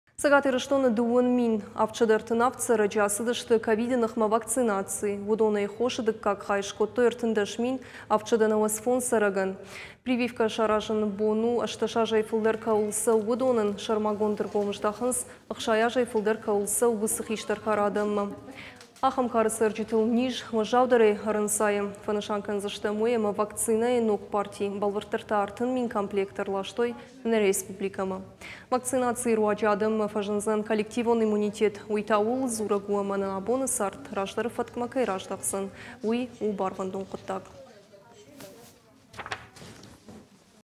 Цæгат Ирыстоны 20 мин 737 цæрæджы ацыдысты COVID 19 ы ныхмæ вакцинаци
Цæгат Ирыстоны 20 мин 737 цæрæджы ацыдысты Covid-19-ы ныхмæ вакцинаци. Уыдонæй хосы дыккаг хай скодтой 13 мин 795 цæрæгӕн. Прививкæ саразыны бон у, 18 азæй фылдæр кæуыл цæуы, уыдонæн. Сæрмагонд ӕргом здахынц 60 азæй фылдæр кæуыл цæуы, уыцы хистæр кары адæммæ. Ахæм кары цæрджытыл низ уæззаудæрæй ӕрӕнцайы.

Фæнысан кæндзыстæм уый, ӕмӕ вакцинæйы ног парти, бæлвырддæр та 3 мин комплекты, æрластой нæ республикæмæ. Вакцинацийы 'руаджы адæммӕ фӕзындзӕн коллективон иммунитет, уый та ууыл дзурæг у, æмæ нæ абоны цард раздæры фæткмæ кæй раздӕхдзӕн. У барвæндон хъуыддаг.
Мы рядом в:
ok.ru/irystontv